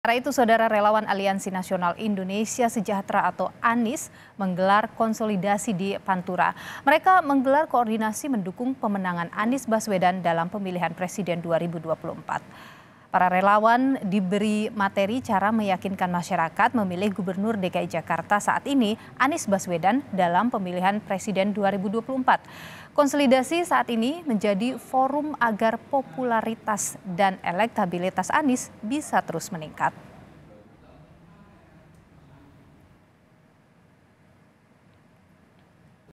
Relawan Konsolidasi Dukung Anies Baswedan Maju Pilpres 2024!
PEMALANG, KOMPAS.TV - Relawan Aliansi Nasional Indonesia Sejahtera atau Anies menggelar konsolidasi di Pantura.

Mereka menggelar koordinasi mendukung pemenangan Anies Baswedan dalam Pemilihan Presiden 2024.

Para relawan diberi materi cara meyakinkan masyarakat memilih Gubernur DKI Jakarta saat ini, Anies Baswedan dalam Pemilihan Presiden 2024.

Konsolidasi saat ini menjadi forum agar polularitas dan elektabilitas Anies bisa terus meningkat.

Baca Juga Pendaftaran Parpol Peserta Pemilu 2024 Dibuka Besok, KPU Gelar Simulasi dan Gladi Resik Hari Ini! di